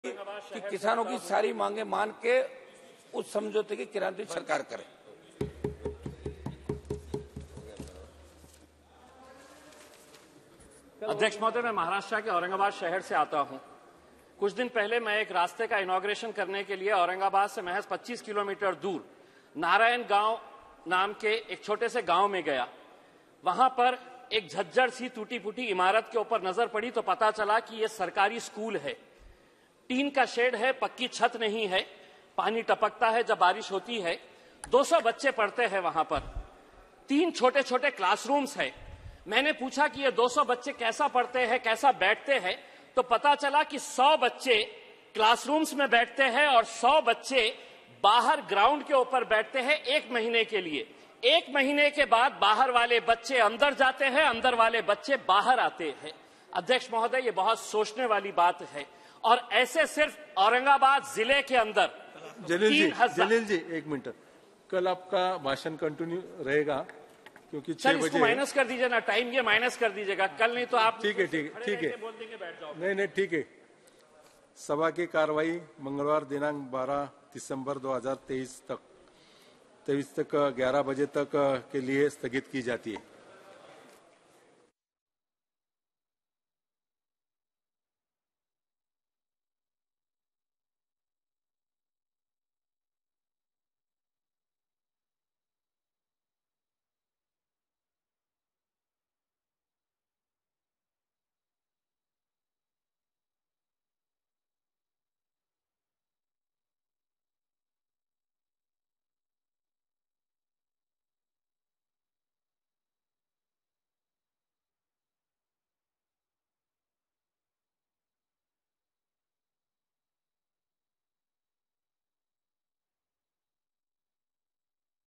Syed Imtiaz Jaleel |Supplementary Demands for Grants 2023-24 & Demands for Excess Grants for 2020-21
Discussion and voting on the Supplementary Demands for Grants - First Batch for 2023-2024  & Demands for Excess Grants for 2020-21 in Lok Sabha.